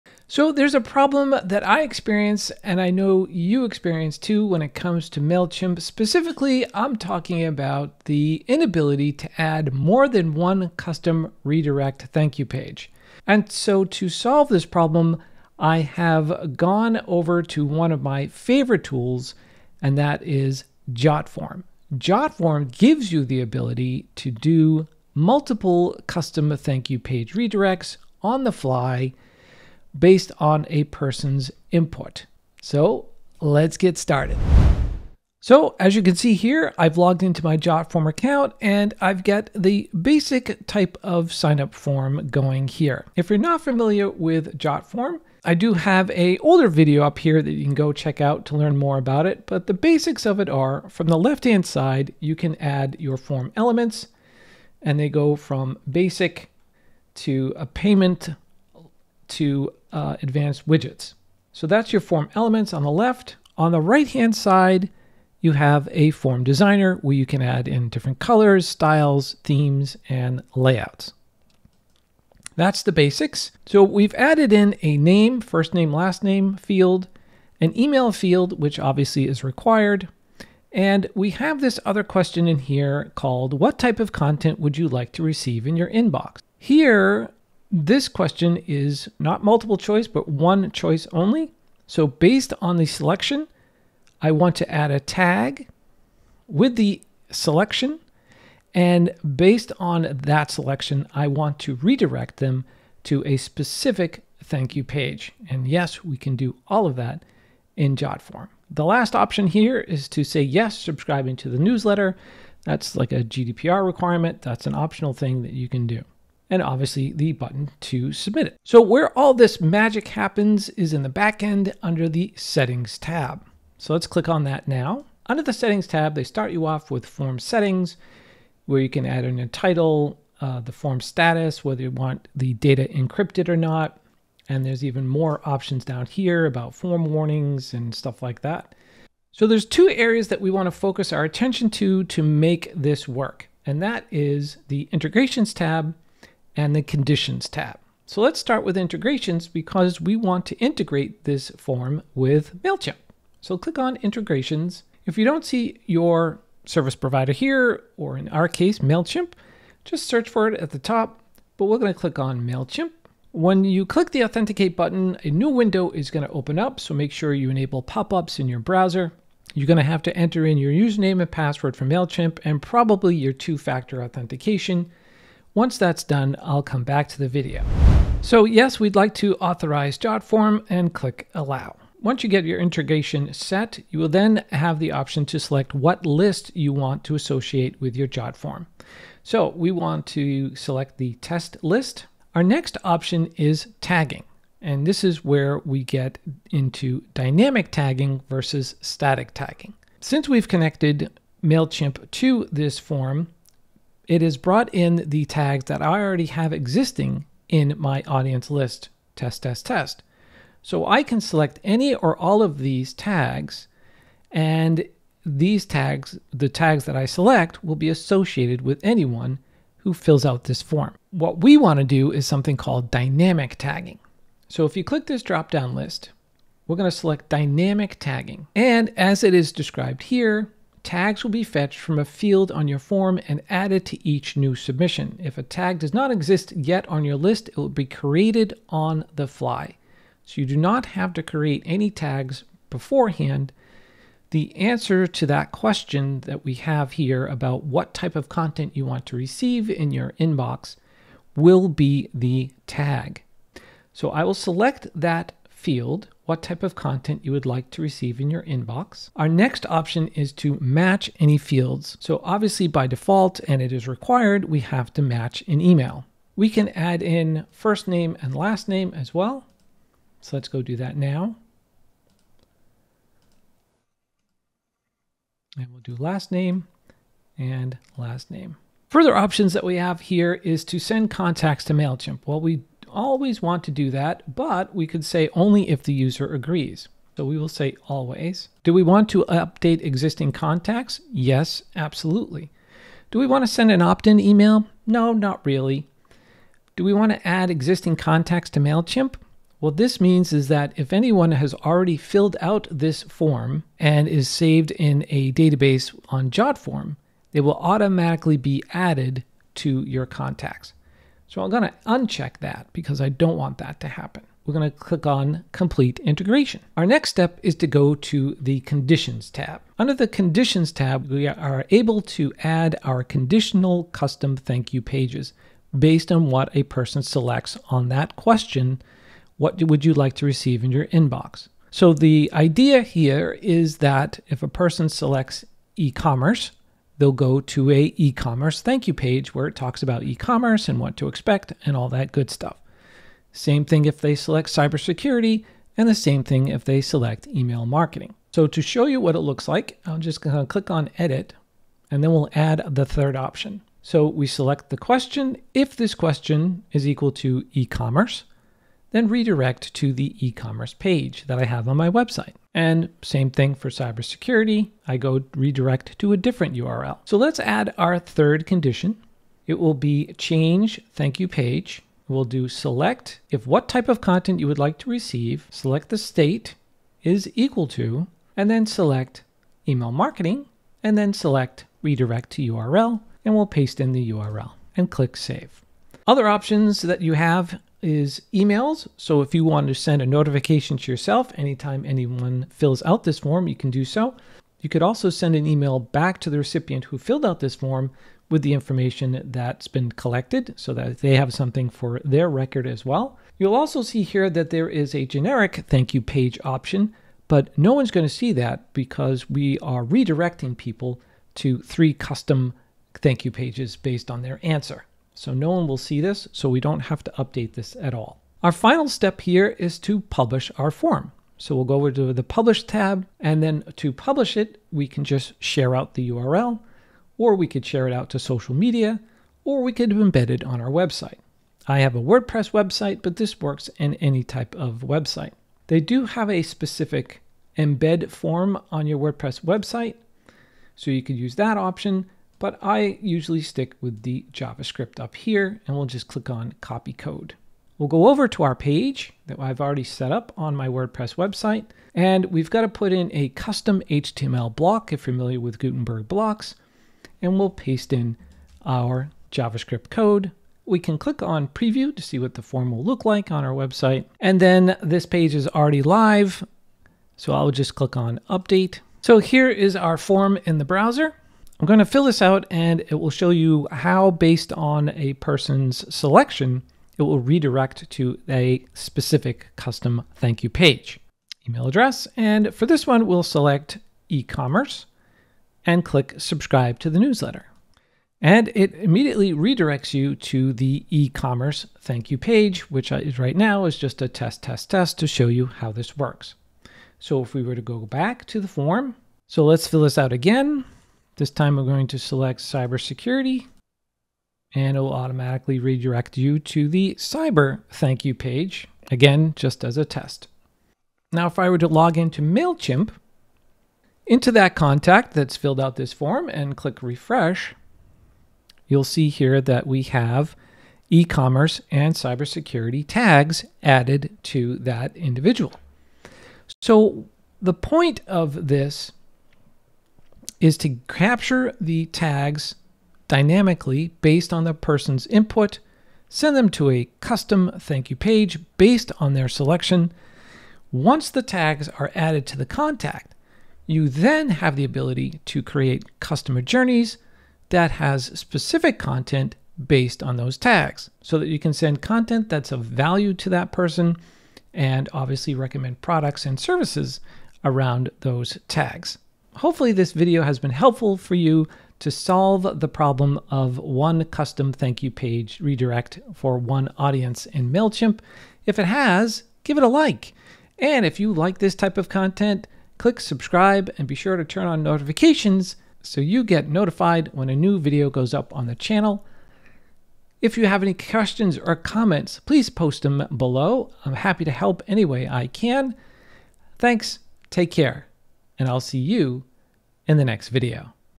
An EASY Way to Create Multiple Thank You Page Redirects for Mailchimp
Mailchimp is limited to using only ONE custom thank you page redirect URL per audience list. In this video, I show how you can create multiple thank you page redirects using one of my favorite tools - Jotform.

In Jotform form settings, you can integrate directly with Mailchimp and easily use Conditional logic workflows to create multiple thank-you redirects.

If this video has helped you in any way, please consider buying me a coffee at

Camtasia: The Best All-In-One Screen Recorder and Video Editor

Chapters